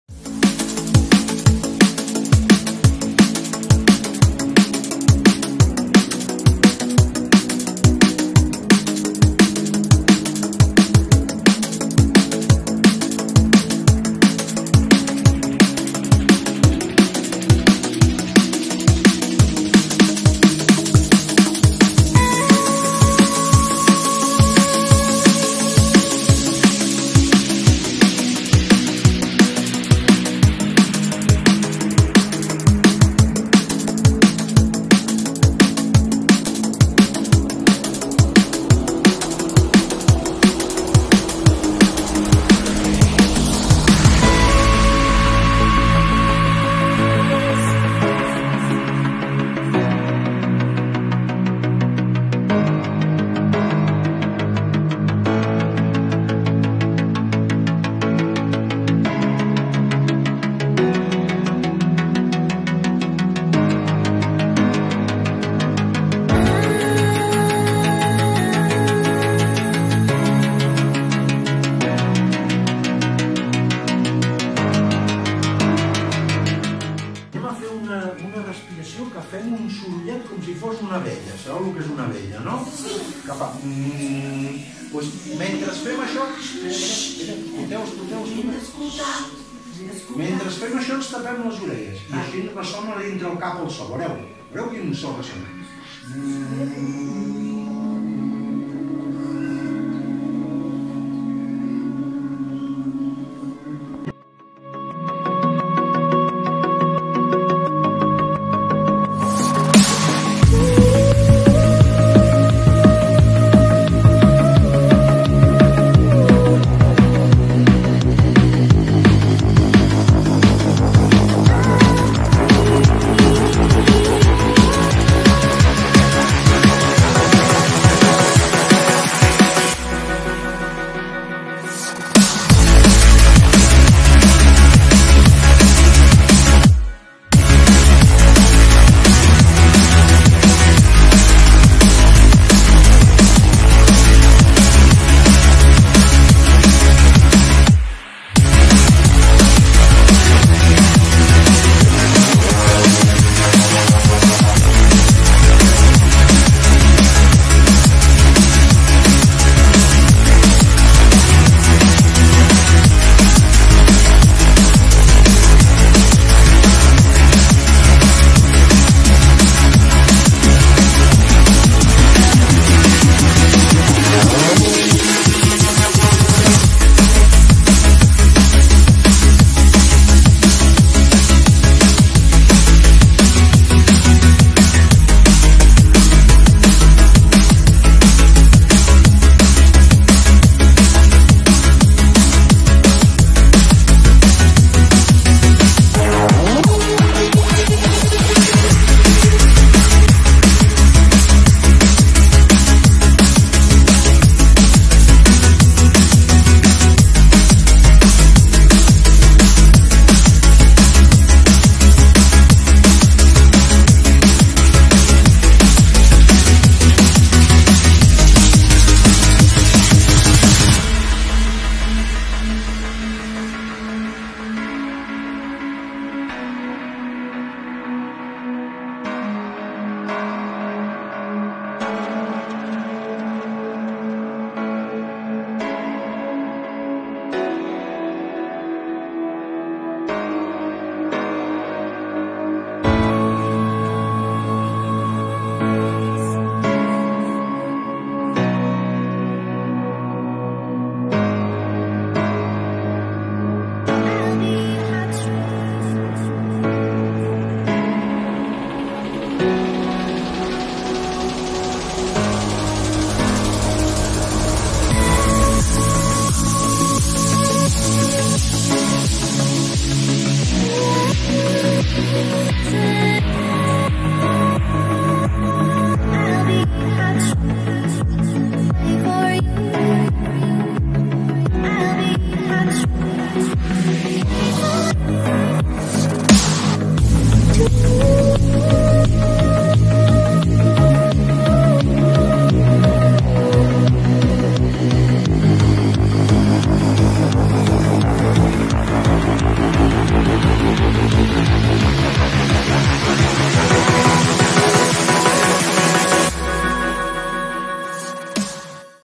Xerrades Oficis'19
El pare de Marcel, la mare del Iago, el pare i la mare de la Natasha i la mare de l'Ian,  dimecres ens van venir a explicar els seus oficis.
Va ser genial!